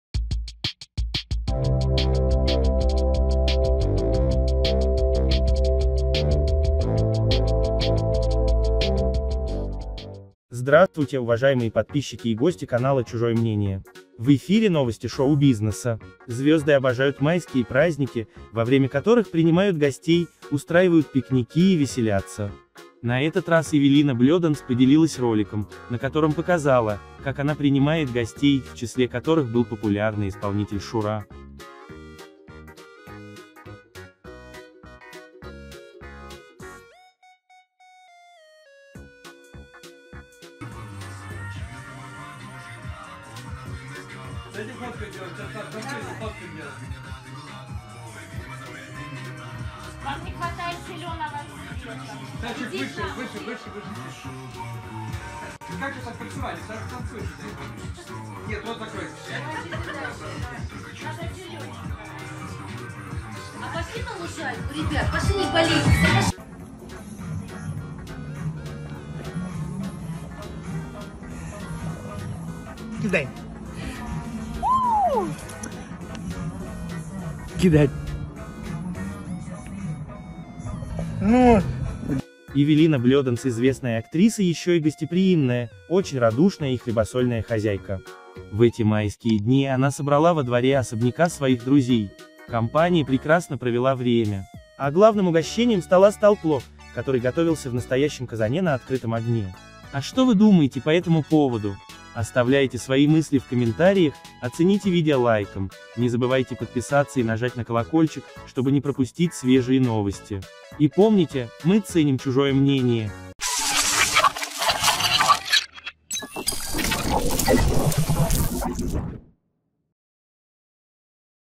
Эвелина Бледанс устроила пикник во дворе своего особняка
Эвелина Бледанс поделилась роликом, на котором показала, как она принимает гостей, в числе которых был популярный исполнитель Шура.